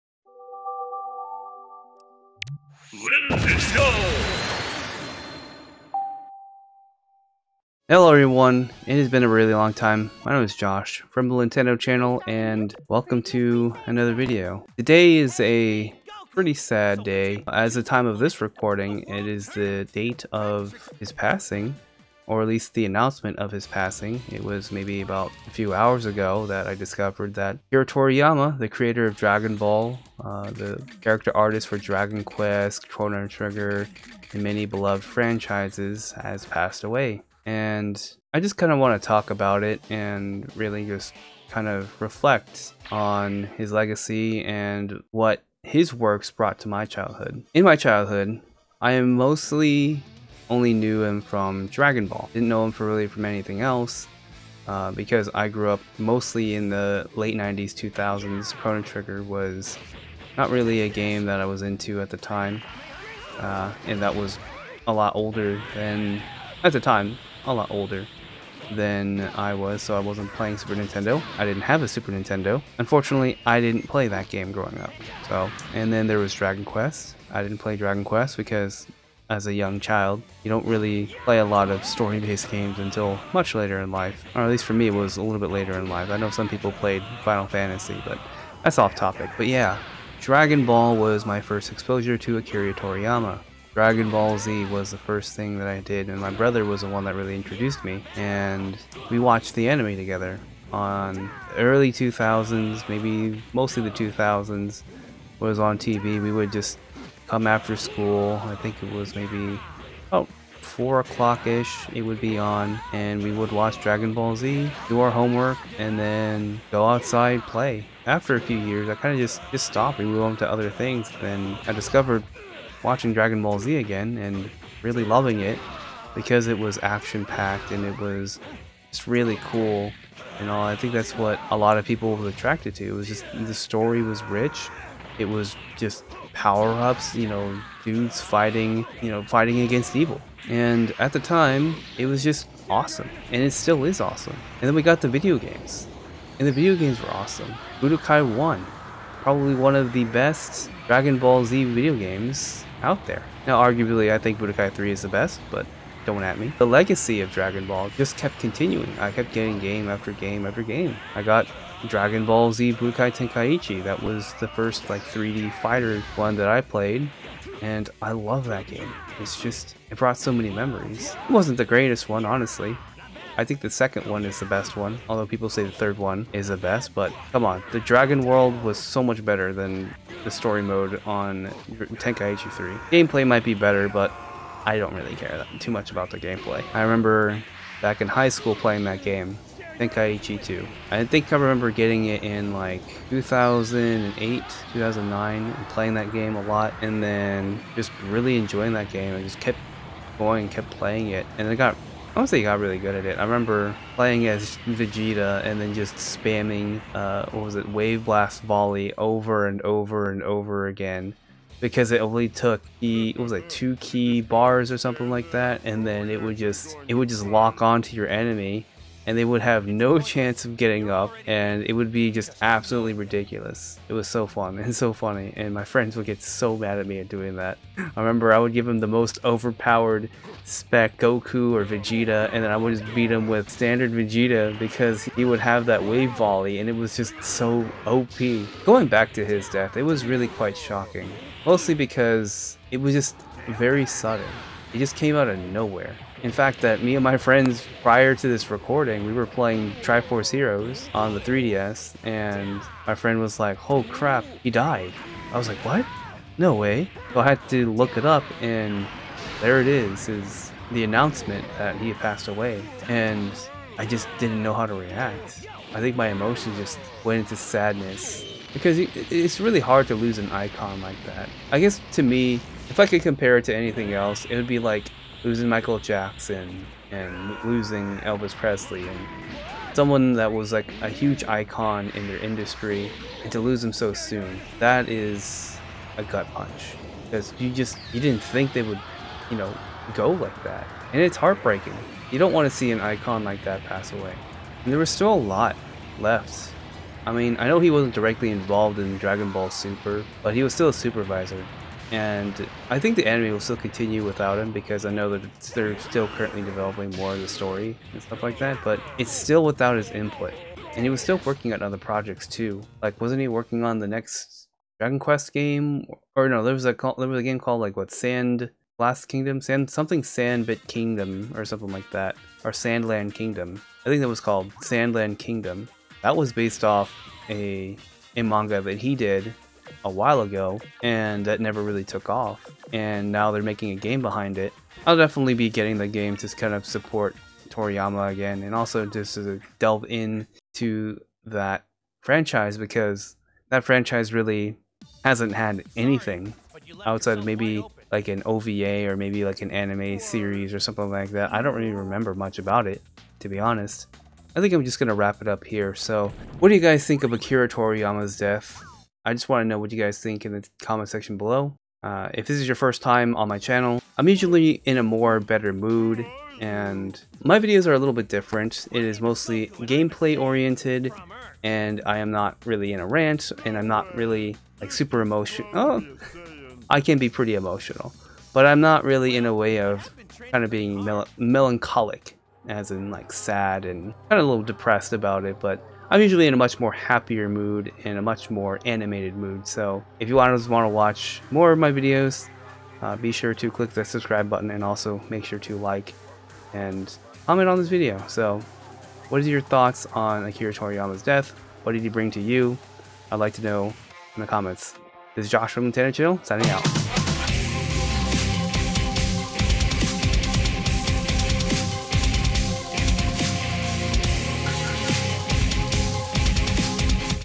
The Impact Akira Toriyama's Works had in My Life
Hello Everyone! This is Josh from the Lintendo Channel. His works had a lot of impact for me as a kid and it was really shocking to hear about his passing.